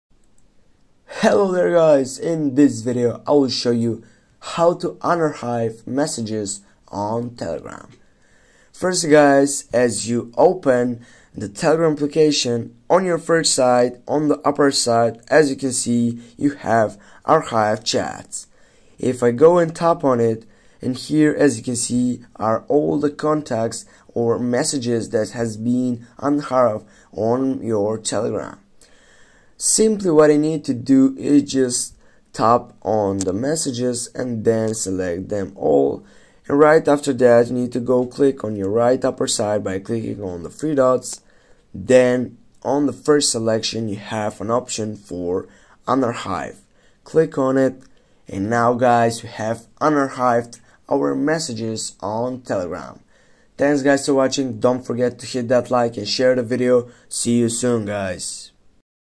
How to Unarchive Messages on Telegram
This video will show you how to unarchive messages on telegram.